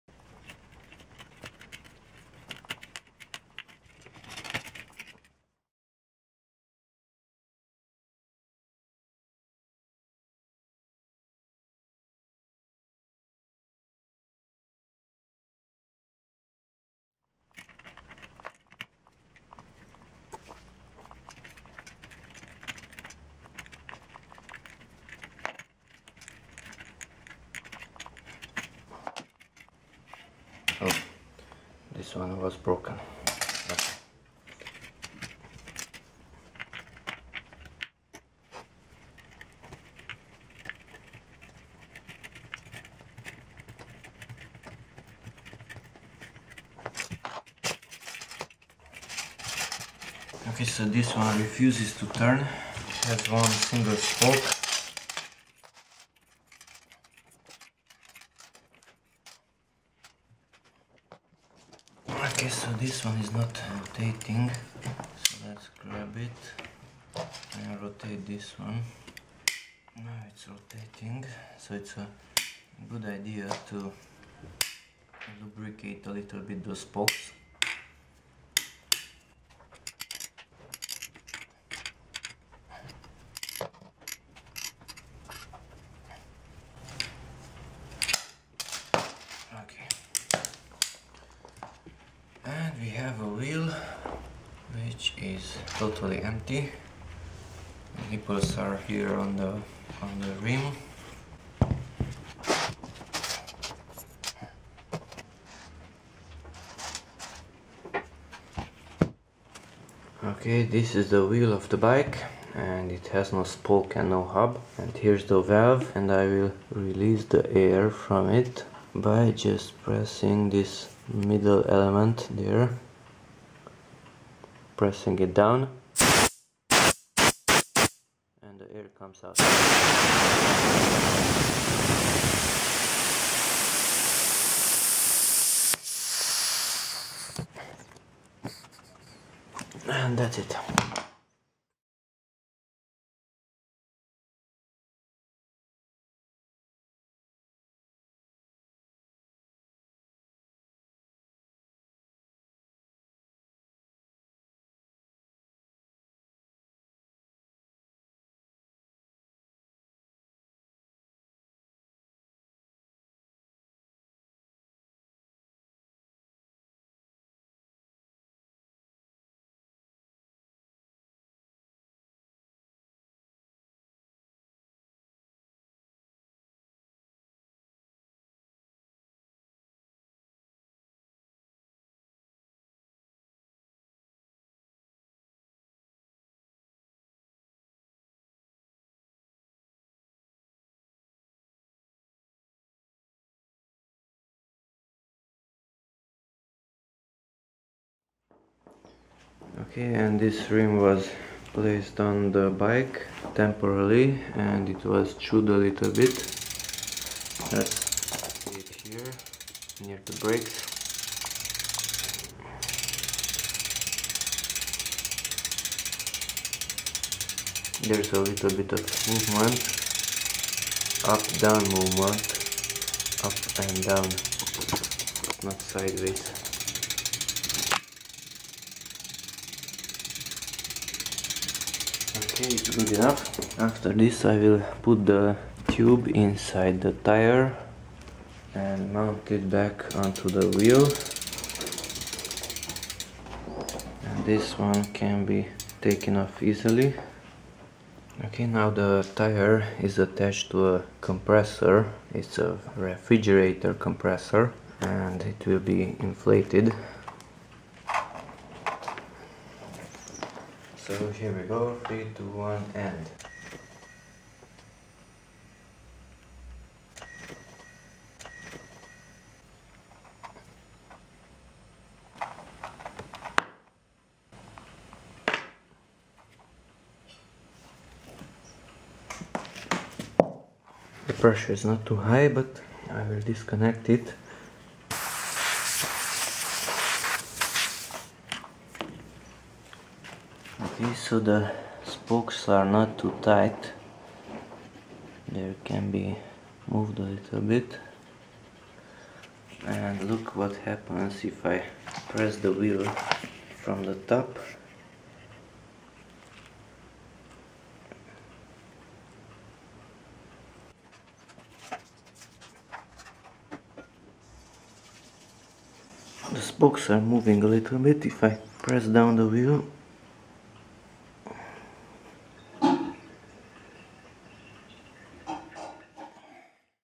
Bike Rim replacement (wheel disassembly)
- taking off the spokes from the hub and the rim
  (it's harder to work near the valve)
- broken spokes are detected
- 1 spoke refuses to come out normally, so it must be grabbed, snapping sounds can be heard, the nipple is almost fused to the spoke
- hub is removed
- tire tube is deflated
- the tire is taken off the rim, than the tube is extracted
- the spokes are held by a tape unto the rim
- a new rim is mounted unto the bike
- the rim nipples are tightened a bit (truing/straightening)
- tube is mounted inside the tire and unto the rim
- refrigerator compressor inflates the tire (visible tire expansion)
- spokes are visibly bending (they were not yet tightened) if weight is applied unto the tire